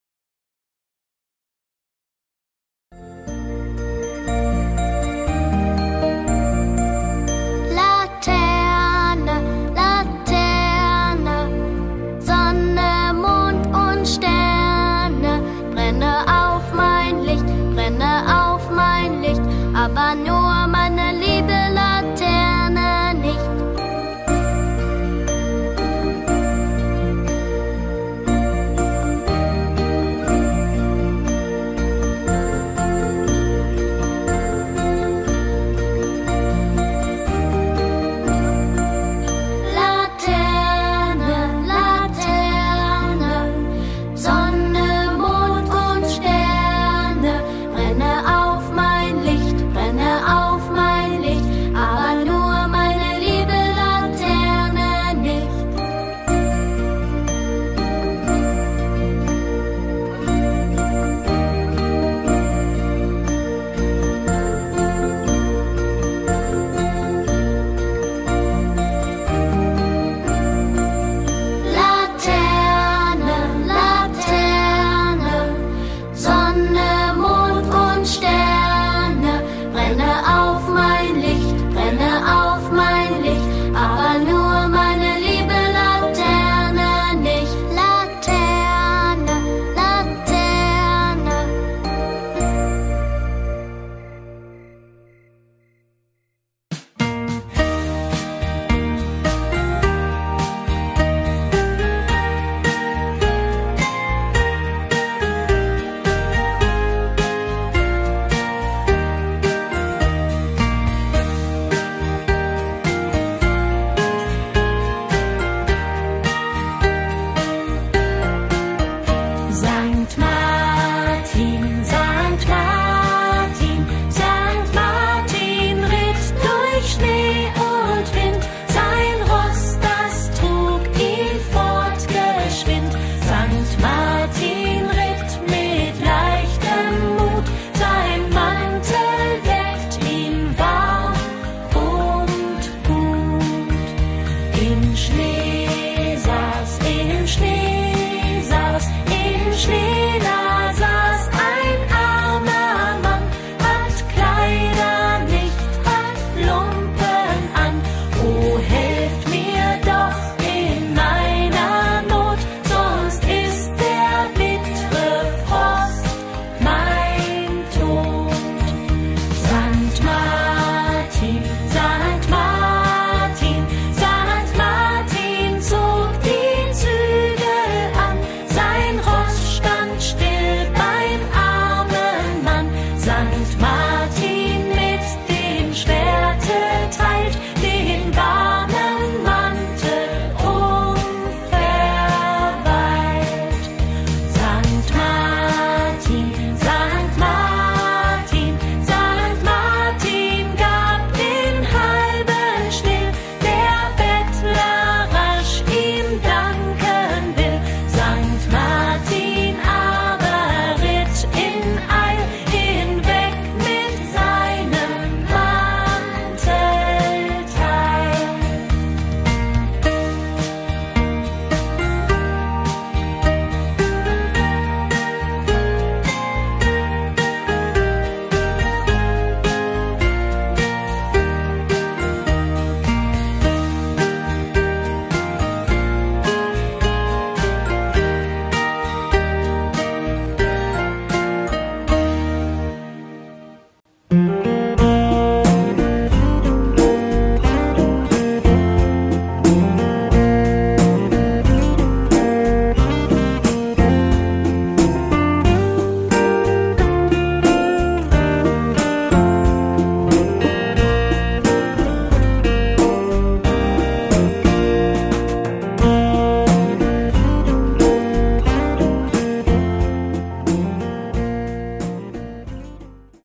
Laternenfest in der Deutschen Schule Athen / «Γιορτή των φαναριών» στη Γερμανική Σχολή Αθηνών
Auch unter Corona-Bedingungen ist es uns gelungen, mit den Kindern in gemütlicher Atmosphäre unser diesjähriges Laternenfest zu feiern. Hier ein kleiner Einblick für Sie. Wir wünschen Ihnen viel Spaß bei Ansehen.
Σε μια ζεστή και κατανυκτική ατμόσφαιρα -και παρά τις συνθήκες πανδημίας- καταφέραμε να γιορτάσουμε και φέτος τη «γιορτή των φαναριών». Στο πλαίσιο αυτό προετοιμάσαμε ένα μικρό βίντεο της γιορτής και ευχόμαστε να το απολαύσετε!